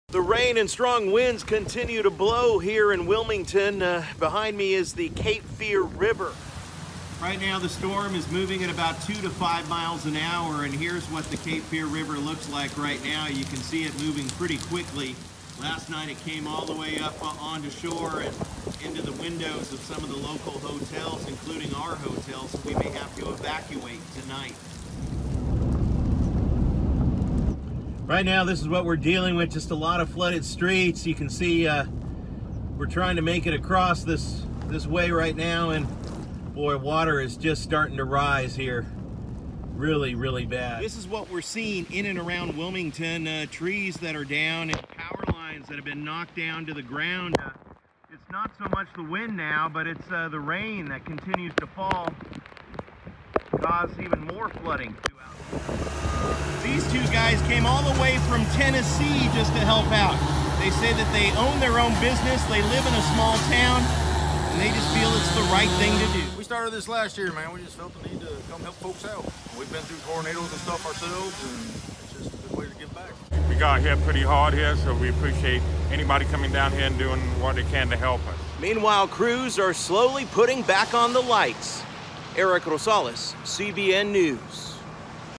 MUST SEE VIDEO: CBN News' Erik Rosales Finds Strangers Helping Strangers in Wilmington, North Carol
MUST SEE VIDEO: CBN News' Erik Rosales Finds Strangers Helping Strangers in Wilmington, North Carolina